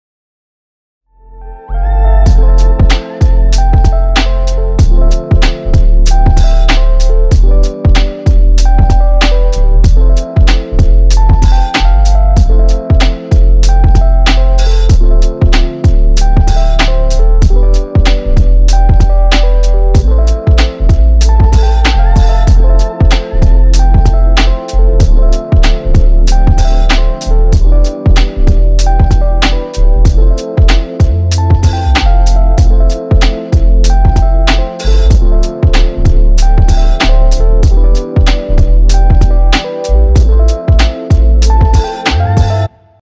Unboxing with Loot Crate - Marvel Gear and Goods "Amazing"
We unboxed the Marvel Gear and Goods "Amazing" crate featuring the three famous webheads! Who is your favorite Spidey character?